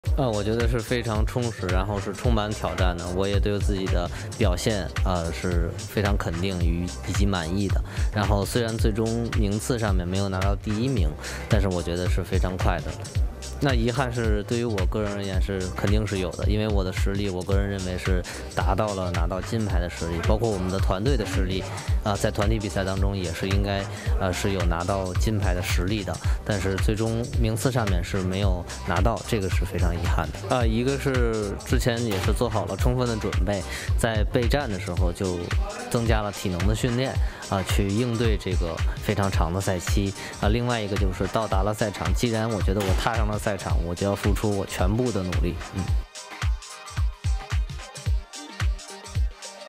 Tokyo 2020 | Exclusive - Xiao Ruoteng happy with his performance despite regrets | 肖若腾独家专访
Chinese gymnastics star Xiao Ruoteng finished his Olympics with one silver and two bronze medals and the 25-year-old said he was happy with his time in Tokyo, but does have some regrets in the men's all-round event.